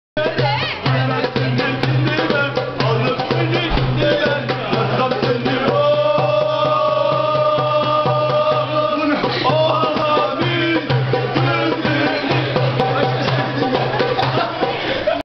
Turkey-Sweden Friendly Game
Turkish students at KTH/Nymble are watching Turkey-Sweden Friendly Game